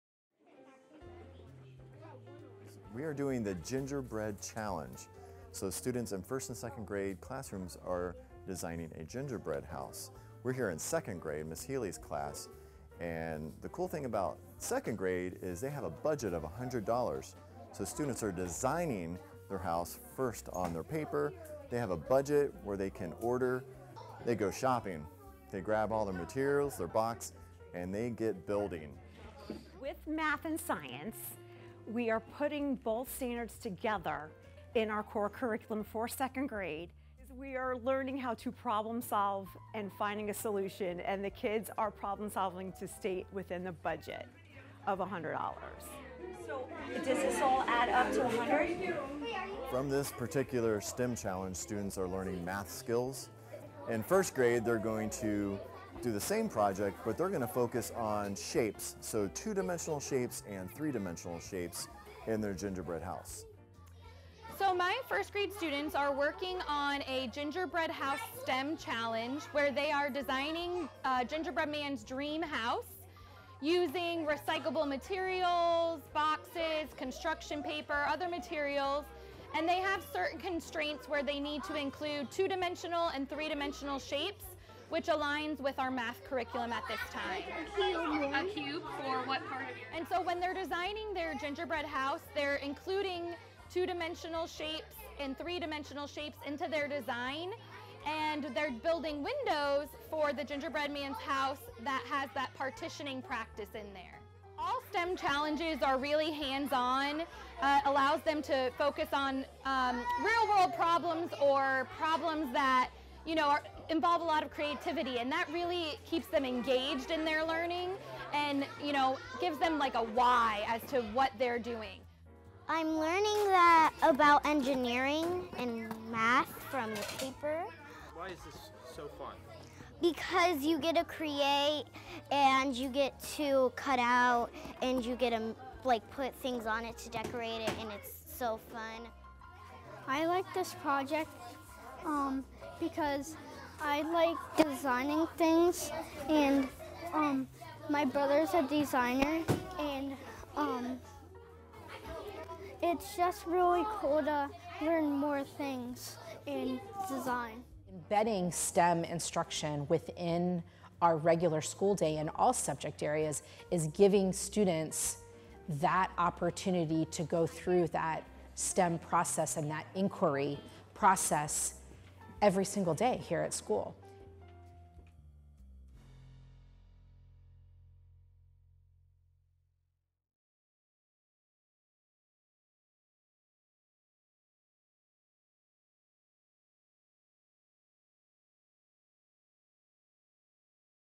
First and second grade students use STEM concepts to build gingerbread houses
Students from Cypress Woods Elementary use STEM concepts to build gingerbread houses from recycled materials. This STEM challenge teaches math skills.